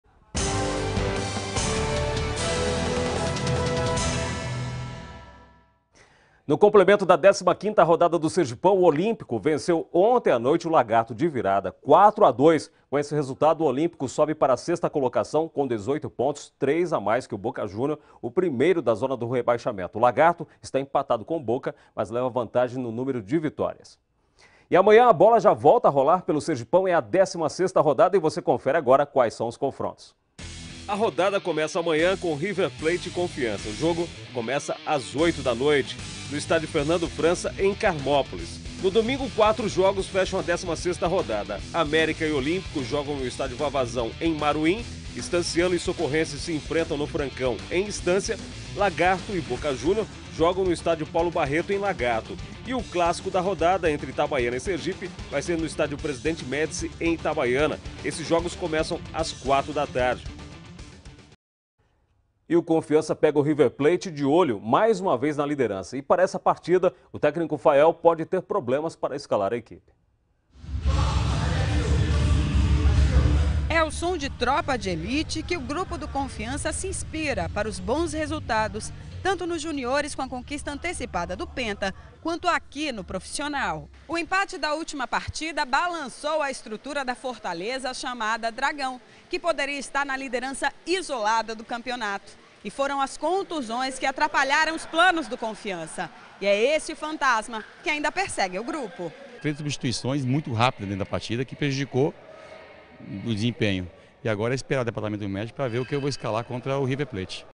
Renato Frota na Academia do Confiança
Treino físico do Confiança na Academia do Dragão